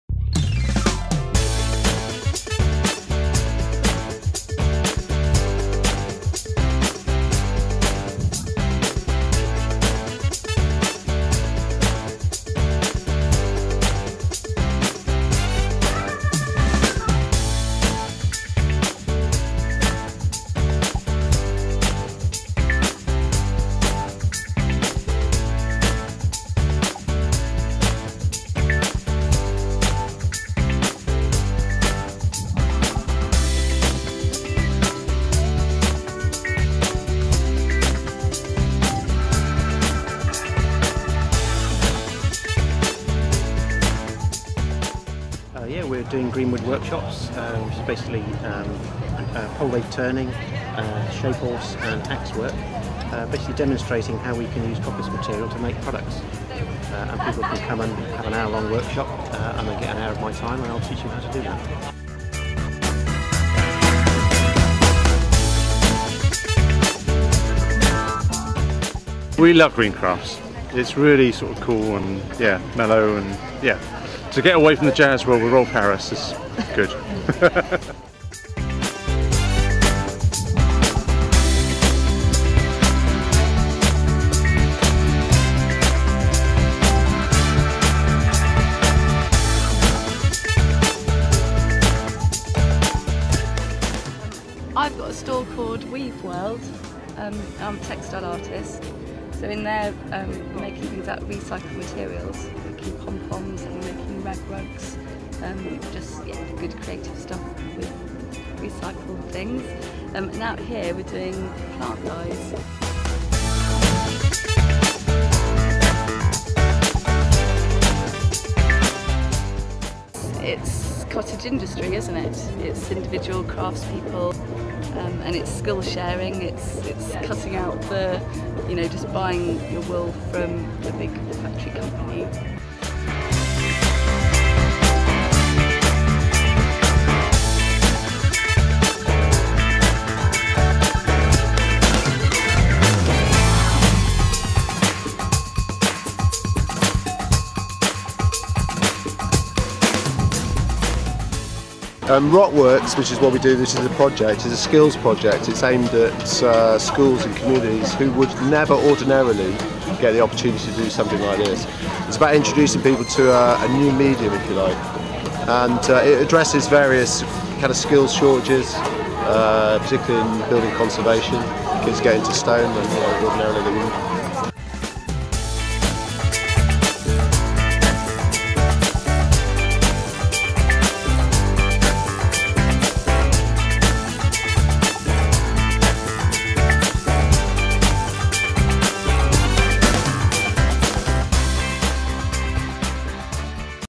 Green Glasto Crafts
green.tv checks out all the green goings on in the crafts field.